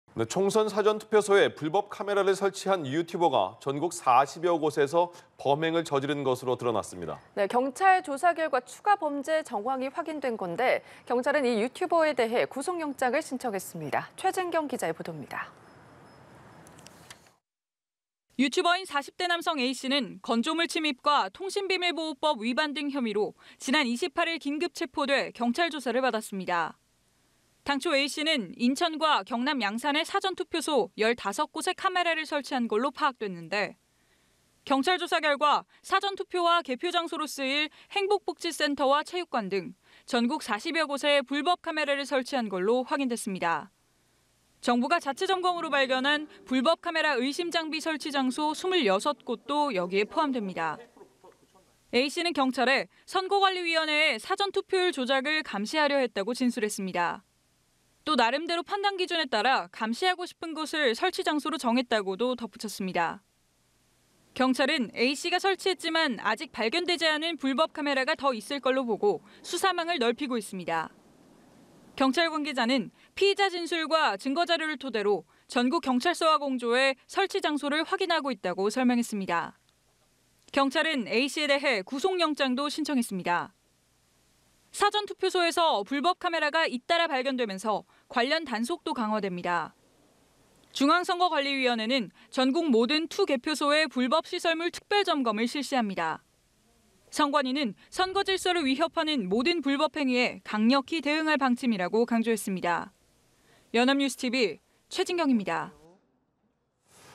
투표소 불법 카메라 40곳으로 늘어…유튜버 구속영장 / 연합뉴스TV (YonhapnewsTV)
투표소 불법 카메라 40곳으로 늘어…유튜버 구속영장
[뉴스리뷰]

[앵커]

총선 사전투표소에 불법 카메라를 설치한 유튜버가 전국 40여 곳에서 범행을 저지른 것으로 드러났습니다.

경찰 조사 결과 추가 범죄 정황이 확인된 건데요.

경찰은 이 유튜버에 대해 구속영장을 신청했습니다.

최진경 기자의 보도입니다.

[기자]

유튜버인 40대 남성 A씨는 건조물 침입과 통신비밀보호법 위반 등 혐의로 지난 28일 긴급체포돼 경찰 조사를 받았습니다.

당초 A씨는 인천과 경남 양산의 사전투표소 15곳에 카메라를 설치한 것으로 파악됐는데,

경찰 조사 결과 사전투표와 개표장소로 쓰일 행복복지센터와 체육관 등 전국 40여 곳에 불법 카메라를 설치한 것으로 확인됐습니다.

정부가 자체 점검으로 발견한, 불법 카메라 의심 장비 설치 장소 26곳도 여기에 포함됩니다.

A씨는 경찰에 "선거관리위원회의 사전 투표율 조작을 감시하려 했다"고 진술했습니다.

또 "나름대로 판단 기준에 따라 감시하고 싶은 곳을 설치 장소로 정했다"고도 덧붙였습니다.

경찰은 A씨가 설치했지만 아직 발견되지 않은 불법 카메라가 더 있을 것으로 보고 수사망을 넓히고 있습니다.

경찰 관계자는 "피의자 진술과 증거자료를 토대로 전국 경찰서와 공조해 설치 장소를 확인하고 있다"고 설명했습니다.

경찰은 A씨에 대해 구속영장도 신청했습니다.

사전투표소에서 불법 카메라가 잇따라 발견되면서 관련 단속도 강화됩니다.

중앙선거관리위원회는 전국 모든 투·개표소의 불법 시설물 특별 점검을 실시합니다.

선관위는 "선거 질서를 위협하는 모든 불법행위에 강력히 대응할 방침"이라고 강조했습니다.